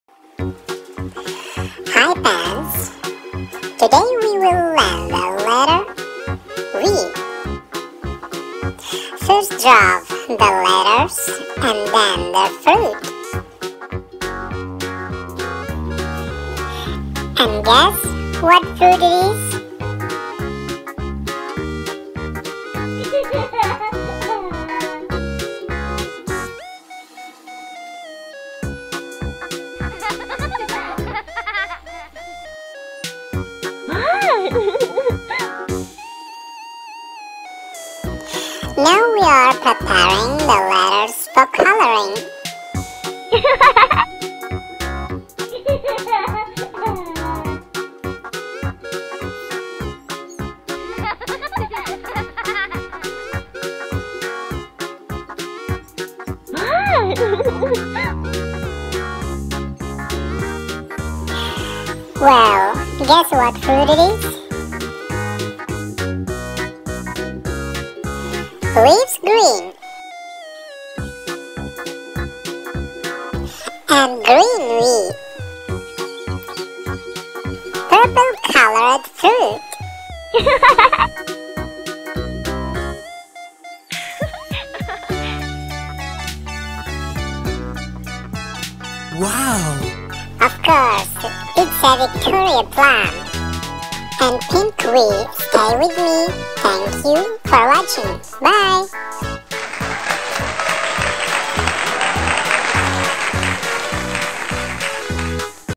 How to draw the letter V for CHILDREN/Fruit draw/Alphabet drawing easy/Step by step
Draw together with the Fox Sisi with fun music)))))